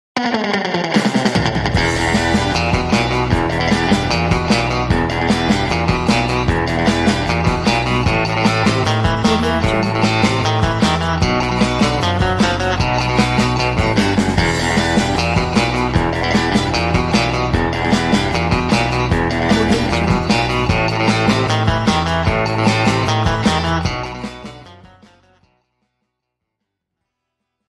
Like Share Subscribe Intro Business Opener
It uses a stunning combination of effects and graphics elements to reveal and enhance your idea. A versatile call to action screen for your Facebook and YouTube videos. Impress your audience with this cool looking and creatively animated AE template. Available in 4K and HD. It contains 9 Images and 12 Text Placeholders!